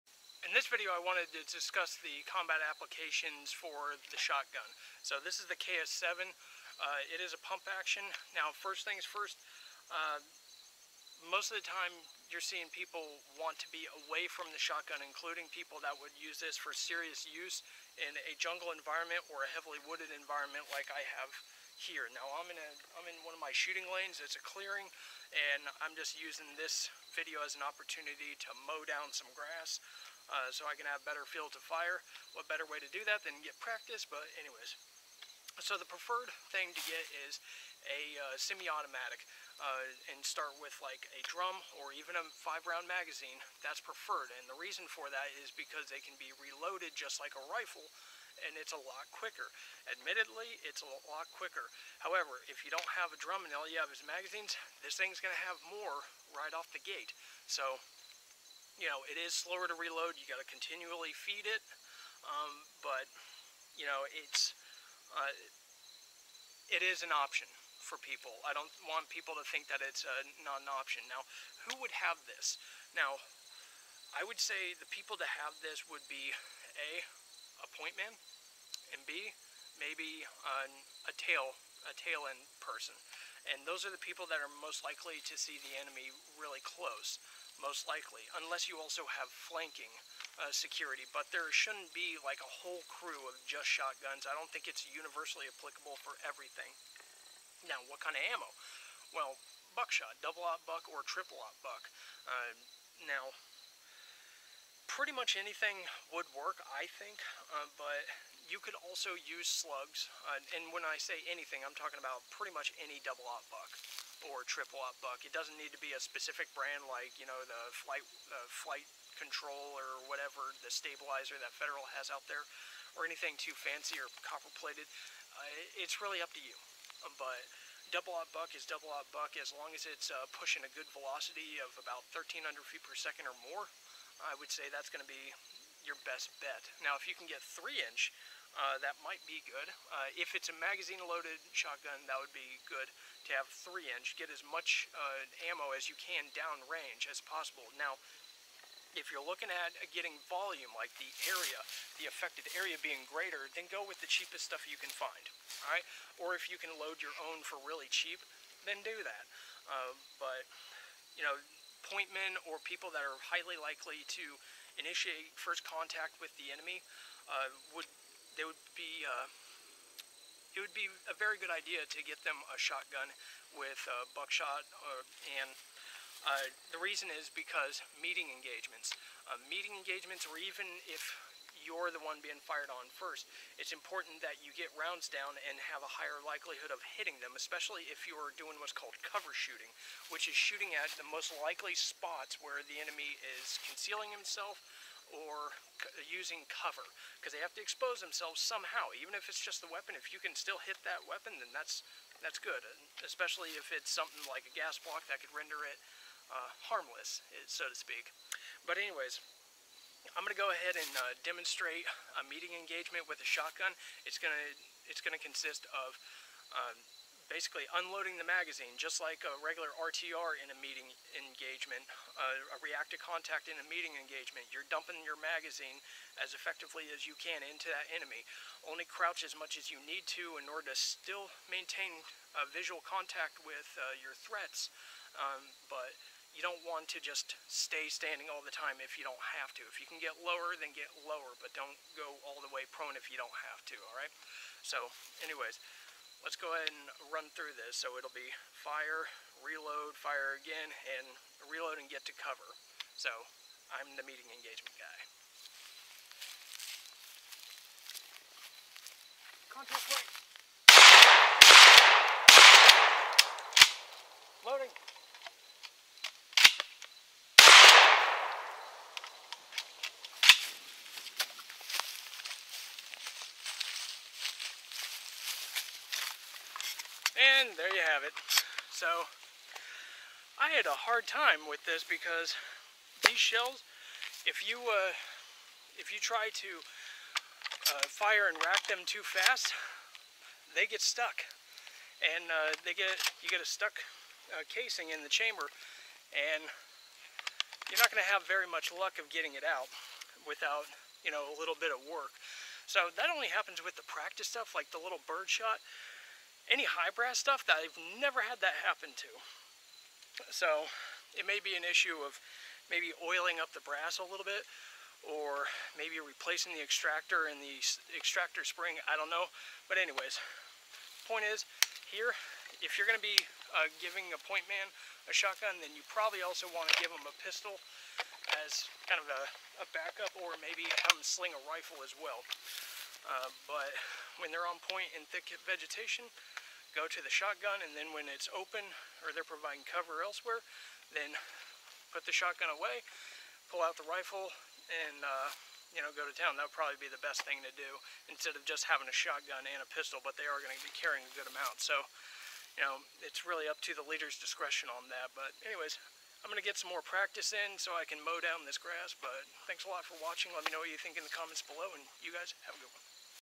Combat Application Shotgun
The shotgun has served warriors for centuries, and still has a place on the modern battlefield. Whether shooting down drones or laying down hate toward a concealed enemy, the shotgun still has a combat application.